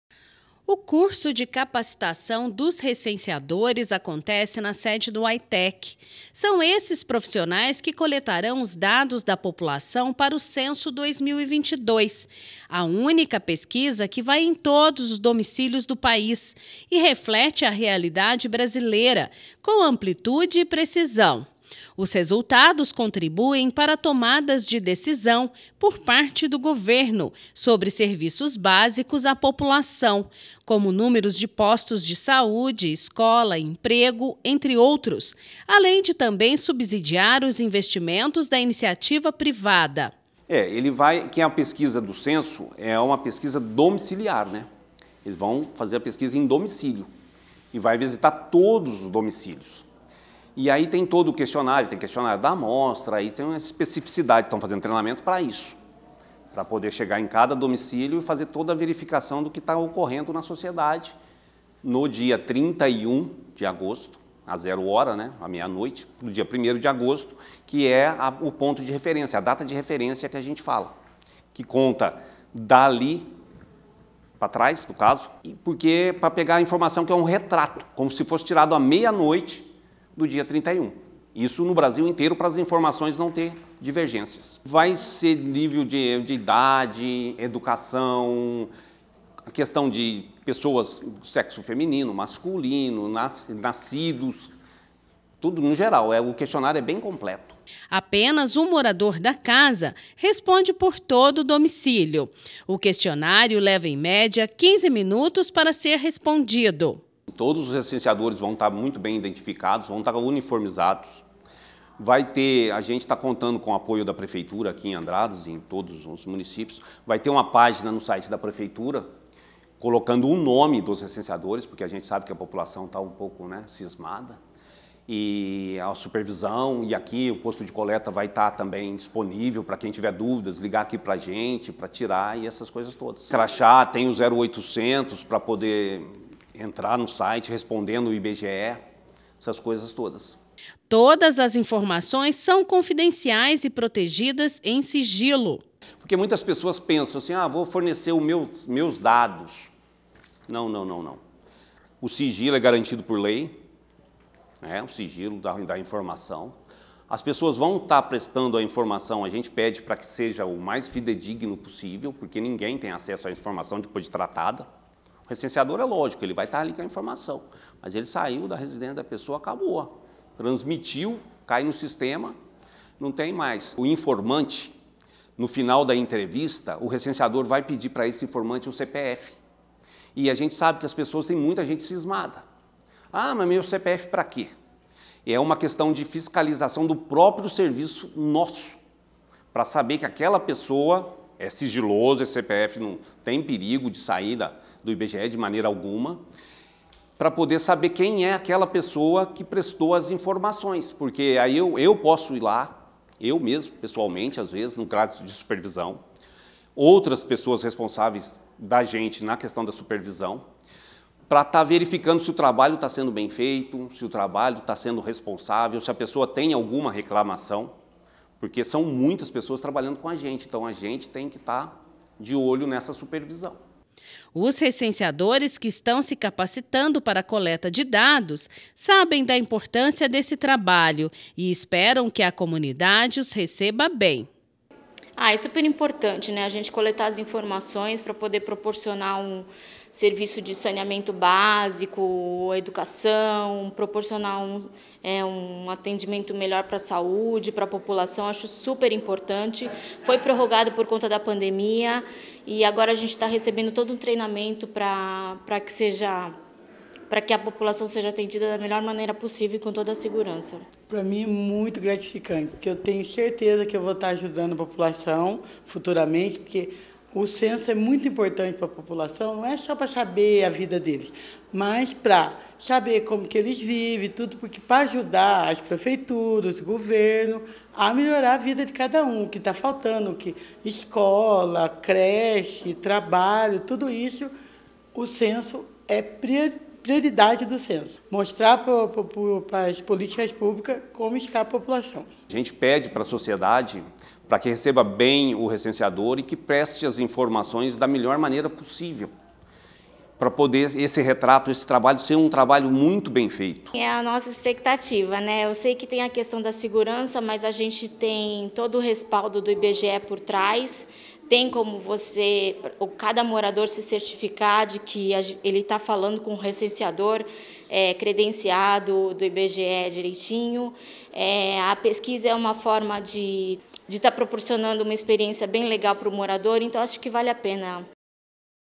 IBGE inicia treinamento dos recenseadores em Andradas
IBGE inicia treinamento dos recenseadores que vão fazer a coleta de dados nos domicílios. O trabalho acontece à partir do dia primeiro de agosto.